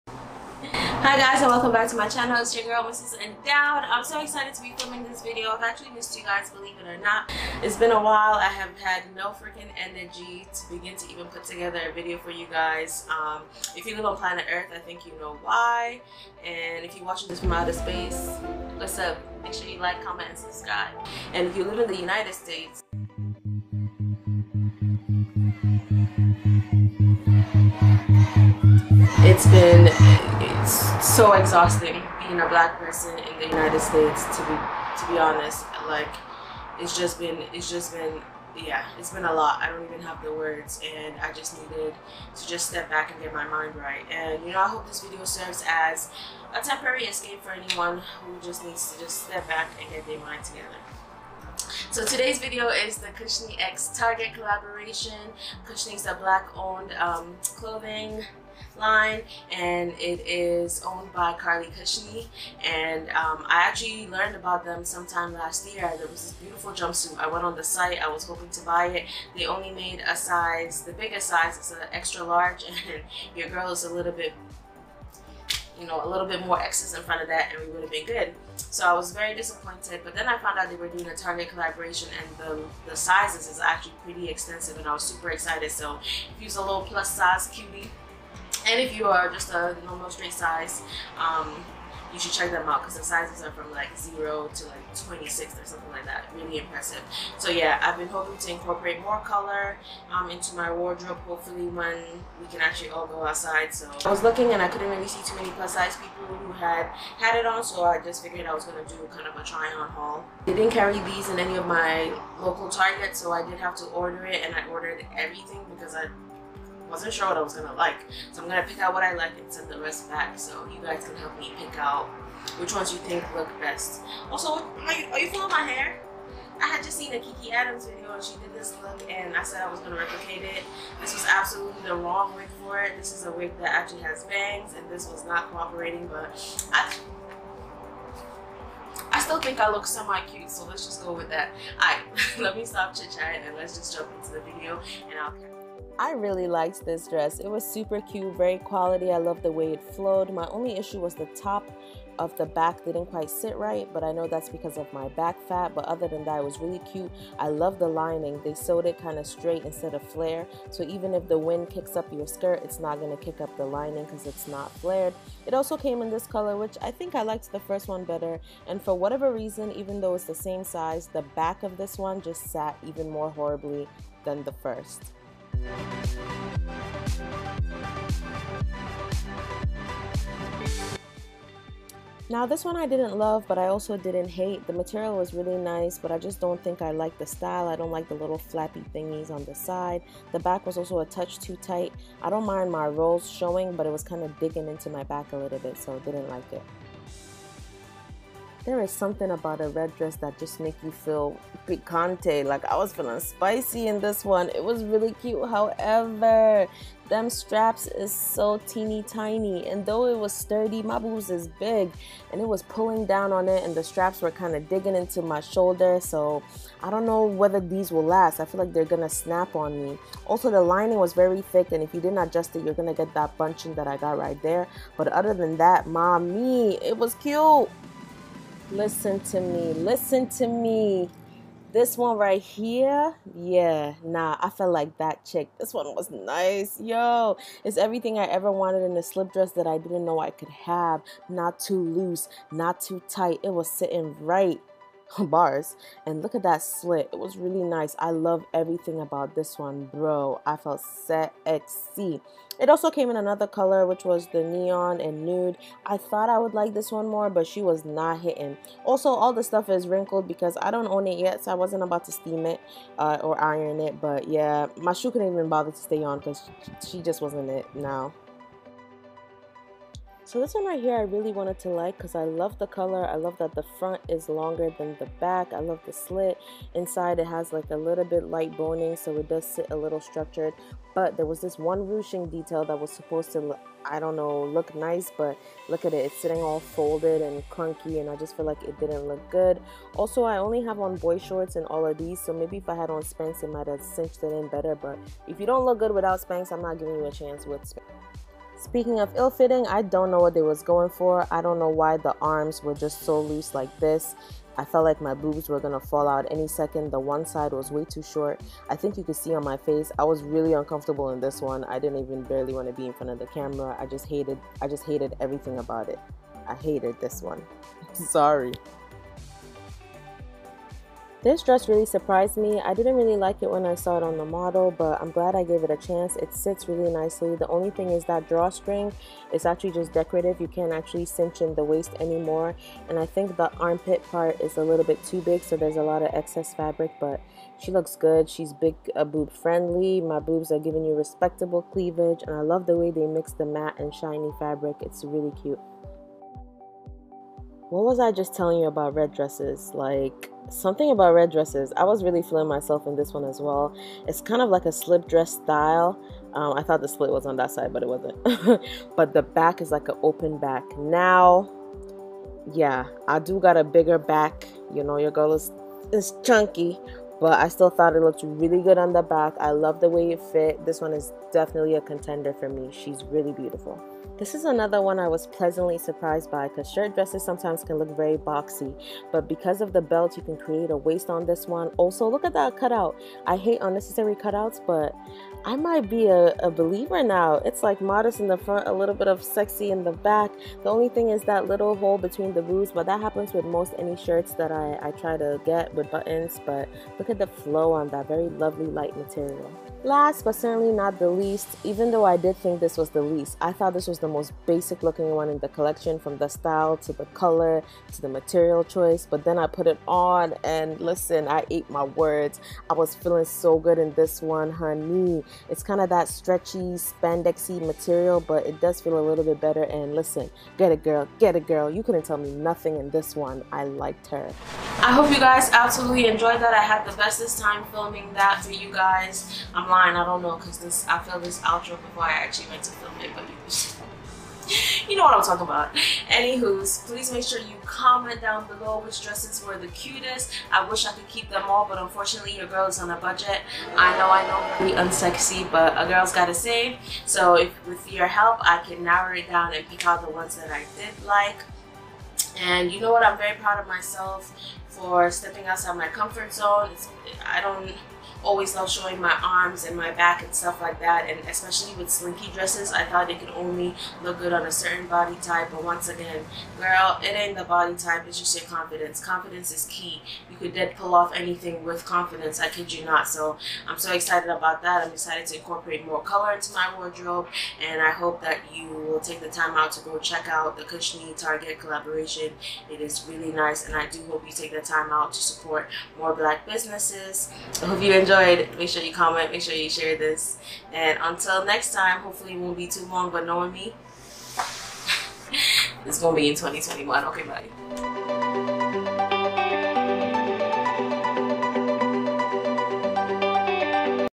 CUSHNIE X TARGET | Plus Size Try On Haul
SO EXCITED TO SUPPORT BLACK OWNED BRANDS/ BUSINESSES & THEIR COLLABORATIONS!
What an awesome collaboration this is. I loved the colors and the quality of the entire collection. At first, I though the prices were a bit too pricey but I think you certainly get your money’s worth. Overall I’m glad I was able to get this video out!

I sincerely apologize about the audio being all over the place, I was having major issues with my microphone and i tried my best to fix it during editing but it was hard.

Link to All Dresses
I got everything in a size 18! or 1X
I'm 5'5 and about 250 lbs for reference!

YALL!! It was no joke filming outside lol NEVER AGAIN!! I hope you enjoyed because so much time and effort went into making it possible. Shoutout to my little brother for being the most patient cameraman ever!!! check out his YT

FEATURED IN VIDEO
(my exact color is sold out but many other beautiful colors are still available)

Mrs.Endowed